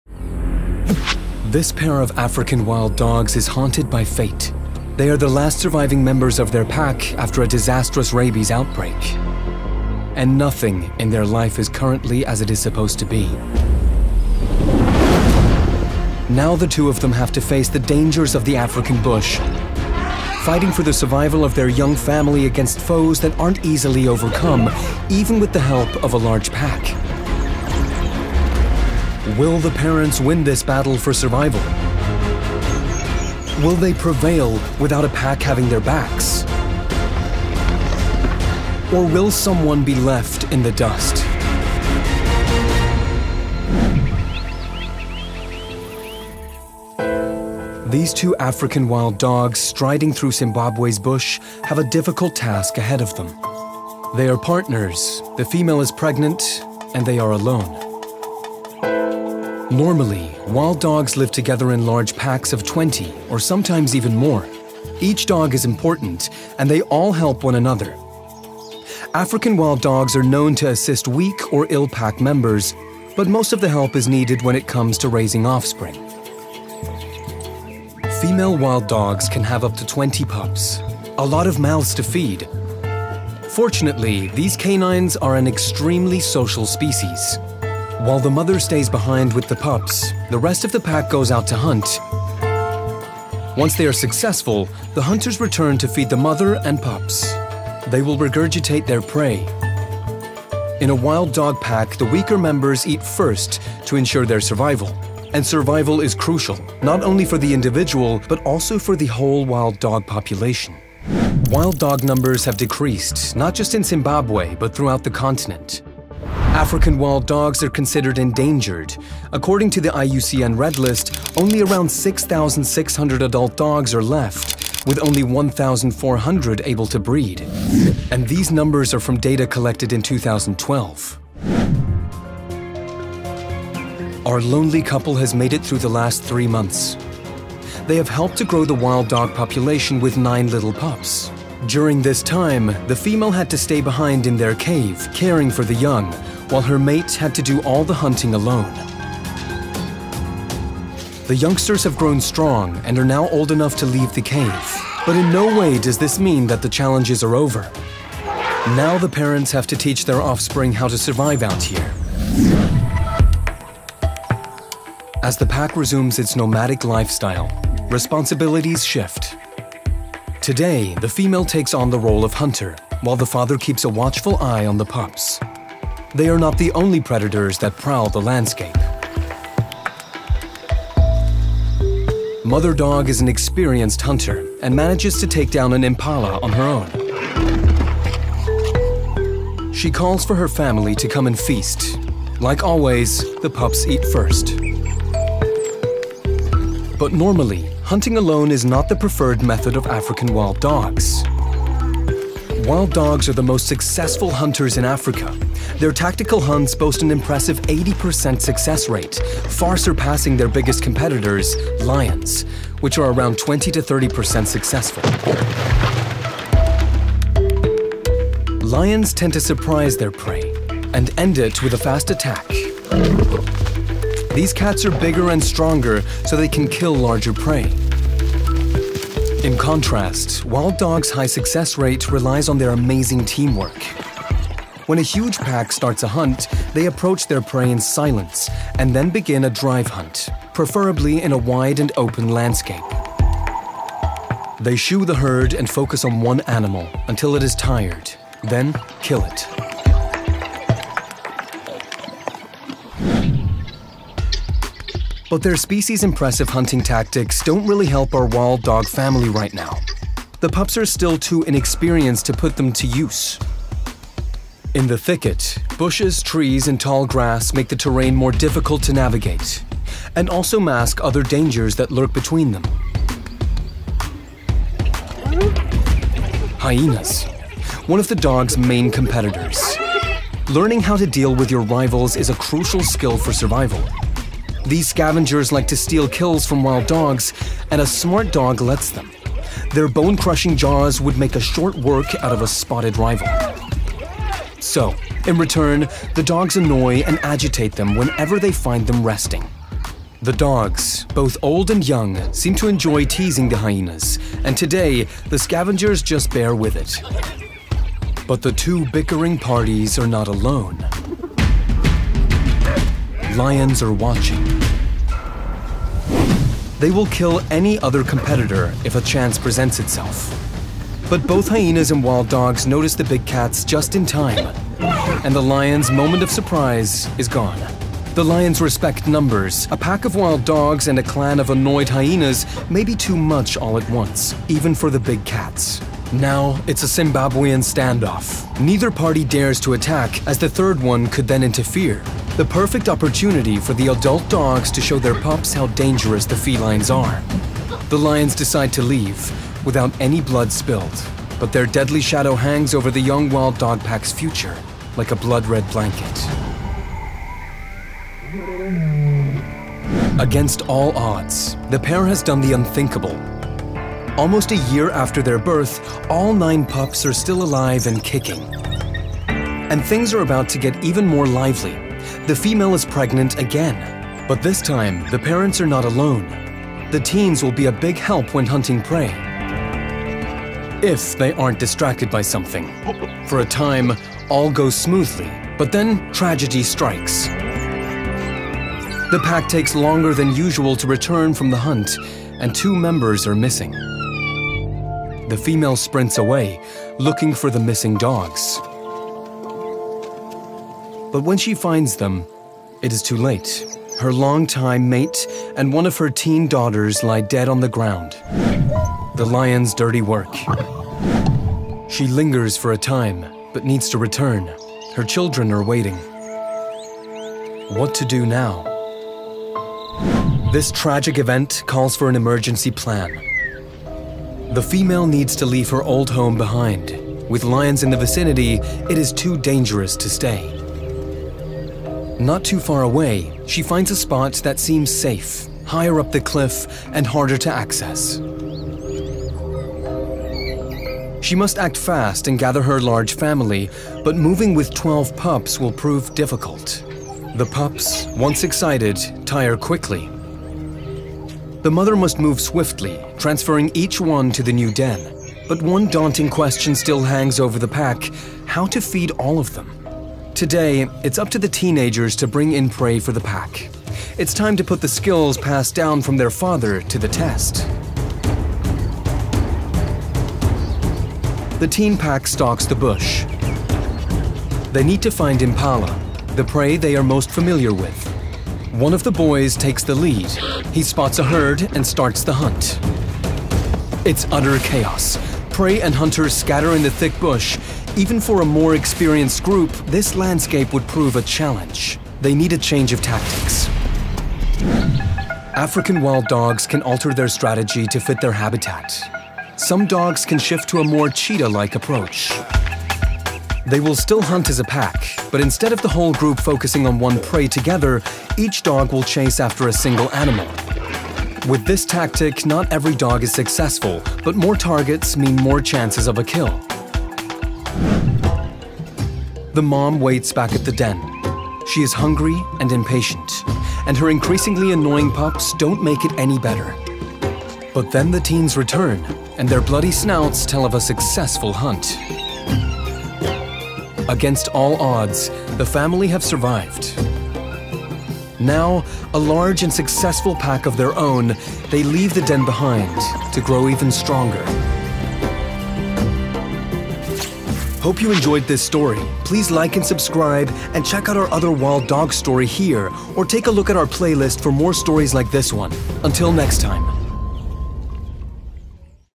Wild Dog Warriors: Thriving Among Lions and Hyenas | Nature is Brutal
African wild dogs are the greatest hunters on the continent, with an incredible 80% success rate. It’s all down to teamwork: these highly social animals live and work in groups of 20 or more.

But wild dogs are endangered. Only 6,600 adults remain in the wild with just 1,400 old enough to breed. And with diseases like rabies decimating packs, the fight for survival is getting tougher. We follow a lone pair of wild dogs through Zimbabwe as they struggle to keep their young family alive.

The last remaining members of a pack decimated by rabies, they must face down lions, hyenas, and sheer starvation in order to stay alive. It isn’t easy – although wild dogs have some advantages over other African predators. They stalk their prey in slow and stealthy teams, and share the work of hunting and raising young. Is one pair of wild dogs enough to build a new pack?